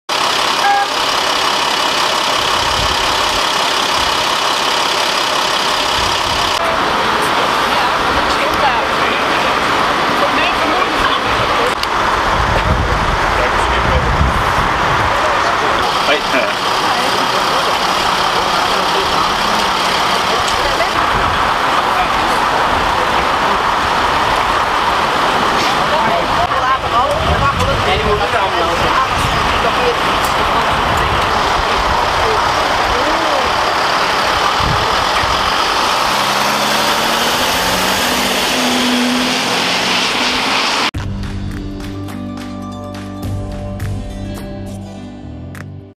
Bestuurder scootmobiel gewond na botsing in Staphorst
Op de Achthoevenweg in Staphorst is zaterdagmiddag een man op een scootmobiel aangereden door een automobilist. De bestuurder van de scootmobiel is met onbekend letsel naar het ziekenhuis gebracht.